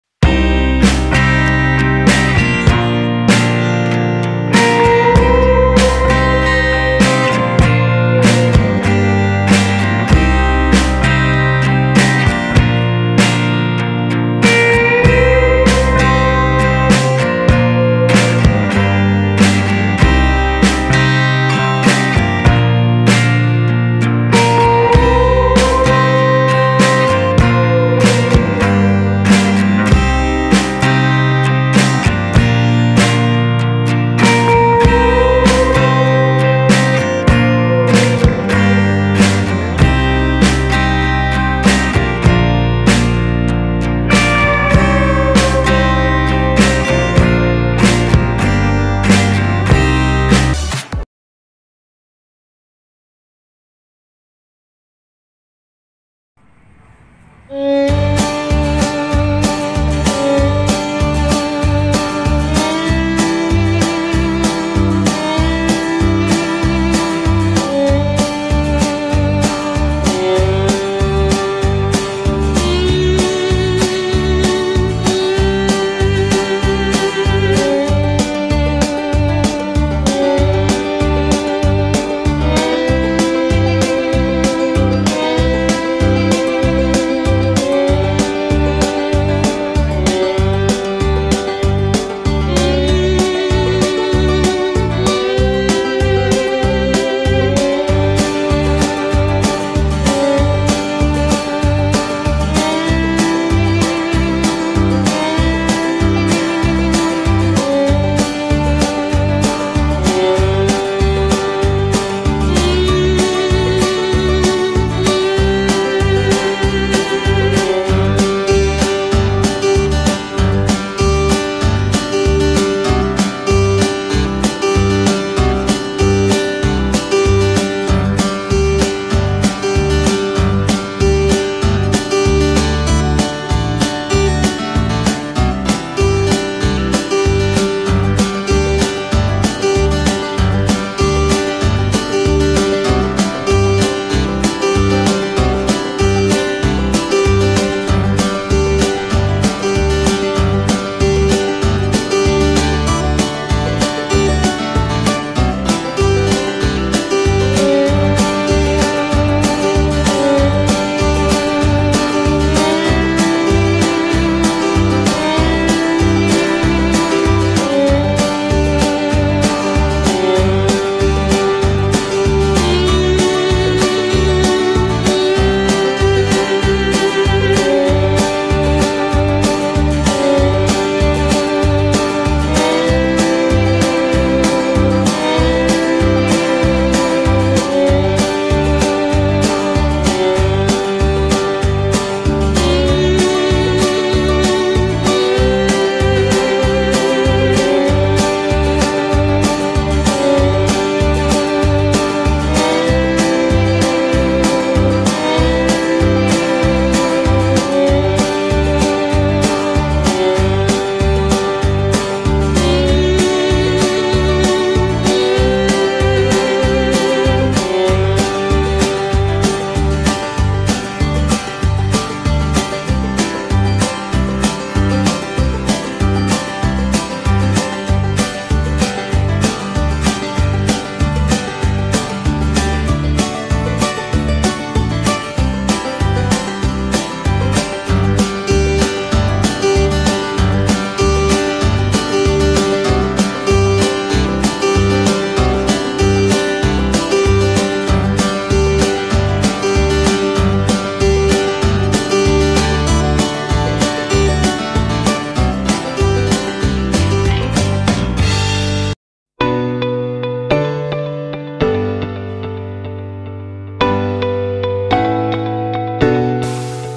BUNGA UNIK || 1 POHON 4 WARNA
Bunga ini sangat menakjubkan dan unik bangeyz, di mana dalam 1 pohon bunganya ada 4 warna. ...